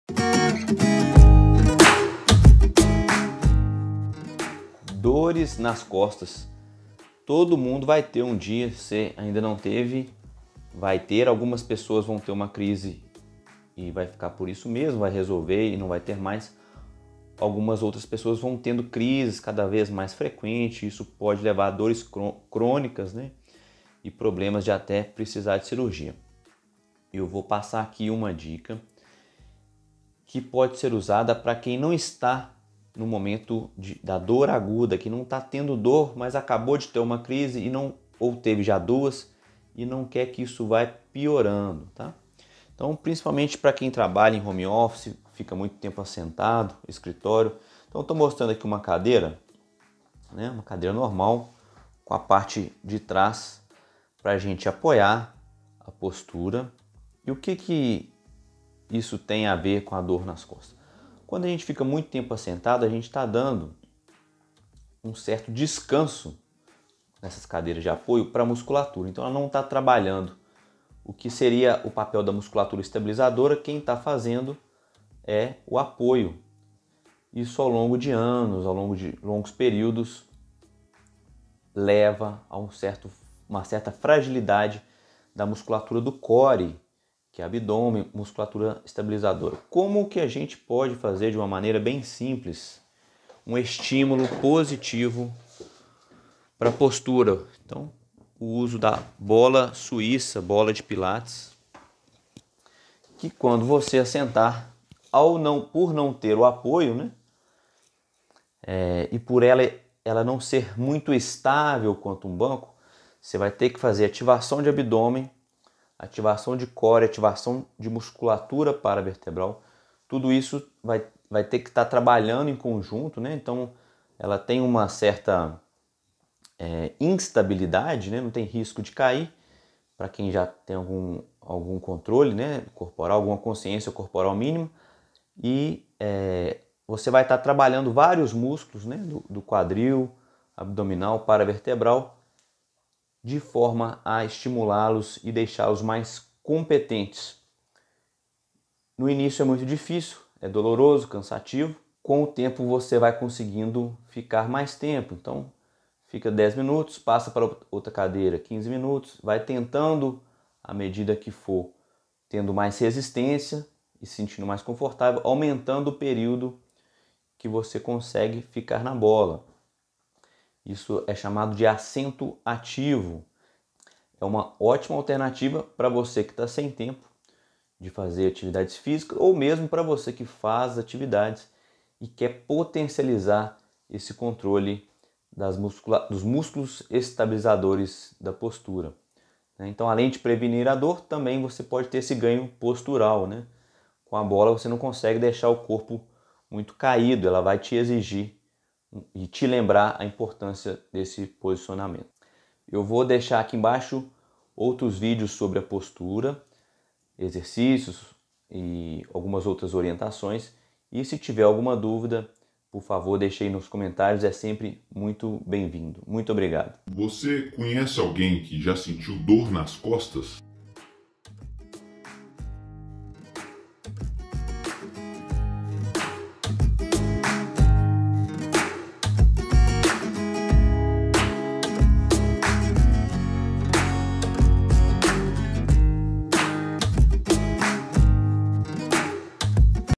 Dor nas costas - músculos paravertebrais e core
✅ Postura e dor nas costas
Assento ativo
Musculatura paravertebral e core
bola suiça ou bola de pilates

💻 Vídeos Relacionados:

Coluna e Postura